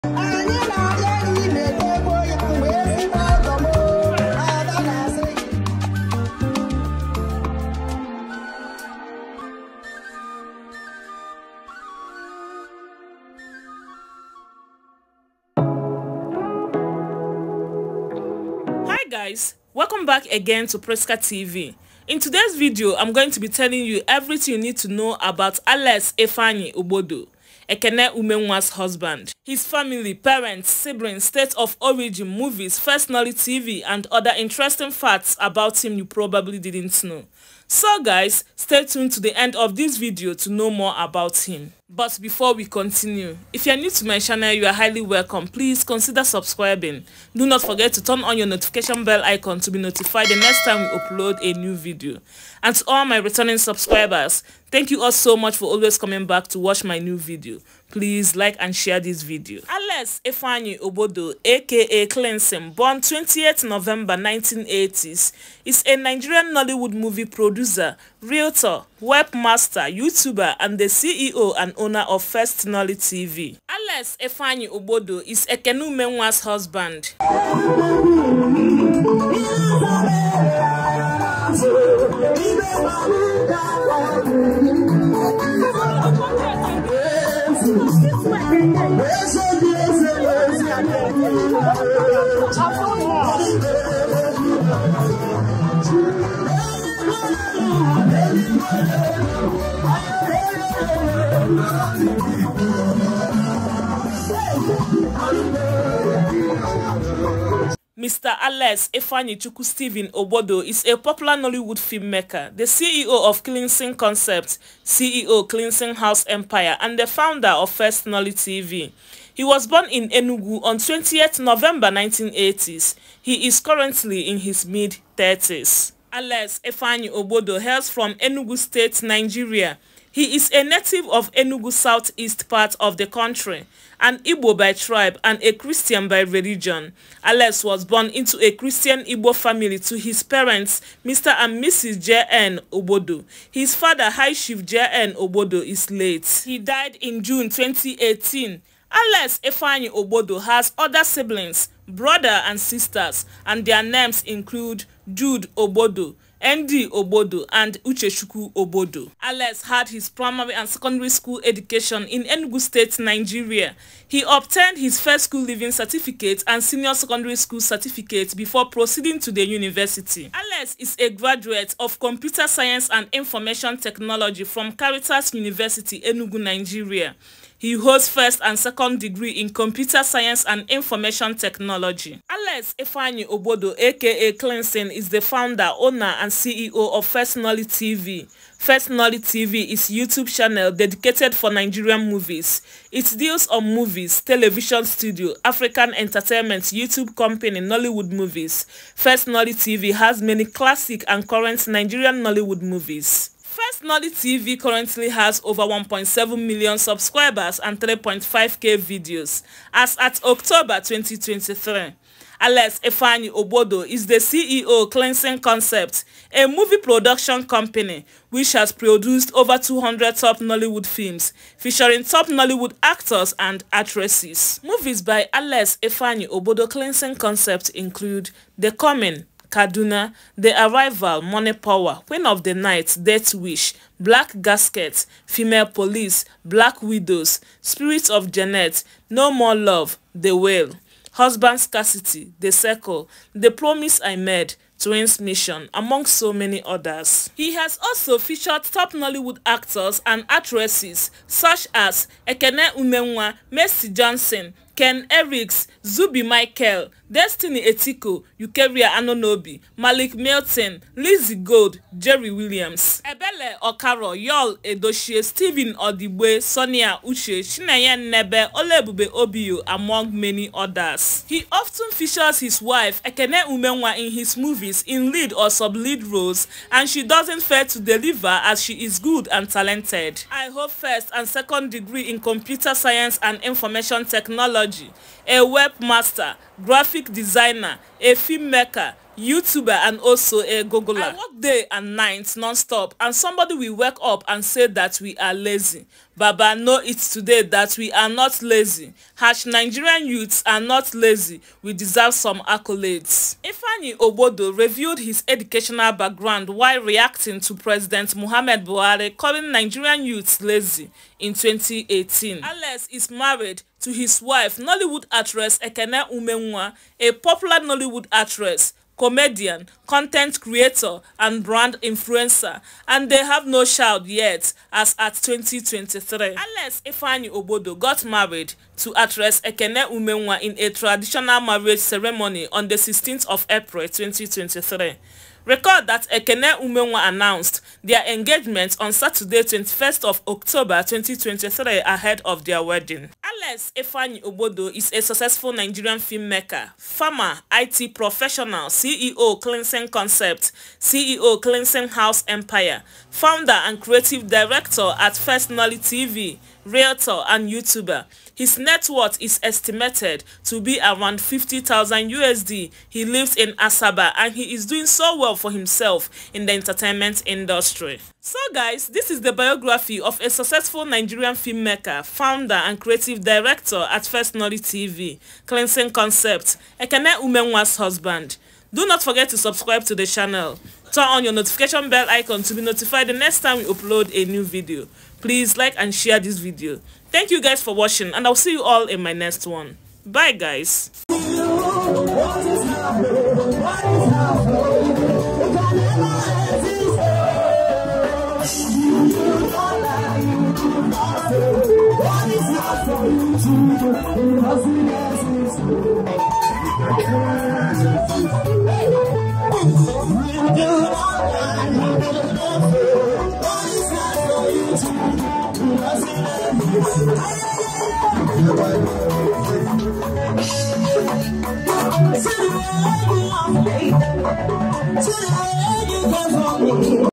ACTRESS EKENE UMENWA HUSBAND KLEANSON BIOGRAPHY FACTS HOUSES, CARS, FAMILY YOU PROBABLY DIDN’T KNOW
KLEANSON concept full biography